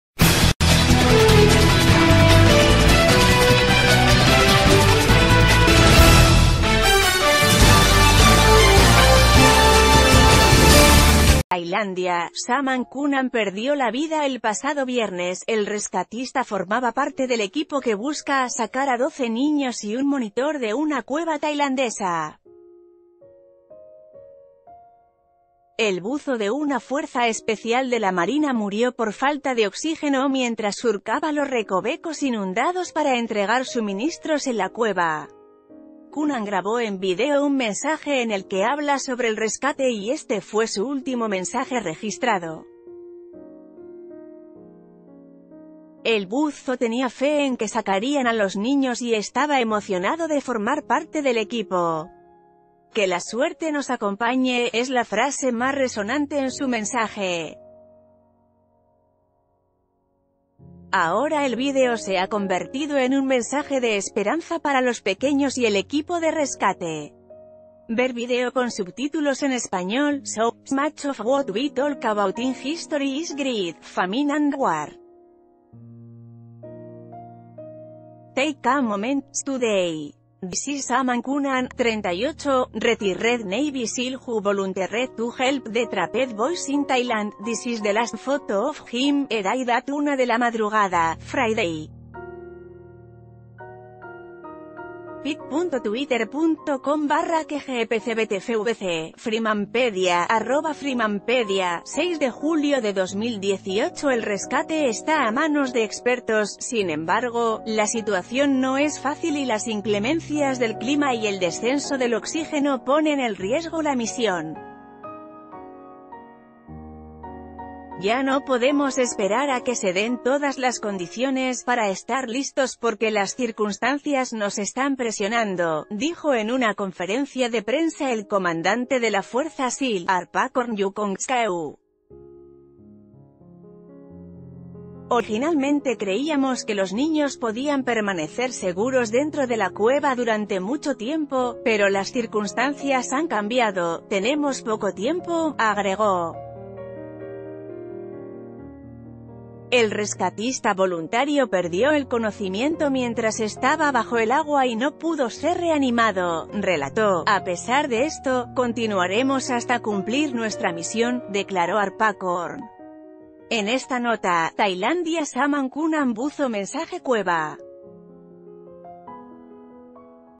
Video El mensaje de buzo antes de morir en cueva de Tailandia
Tailandia.- Saman Kunan perdió la vida el pasado viernes, el rescatista formaba parte del equipo que busca sacar a 12 niños y un monitor de una cueva tailandesa. El buzo de una fuerza especial de la Marina murió por falta de oxígeno mientras surcaba los recovecos inundados para entregar suministros ...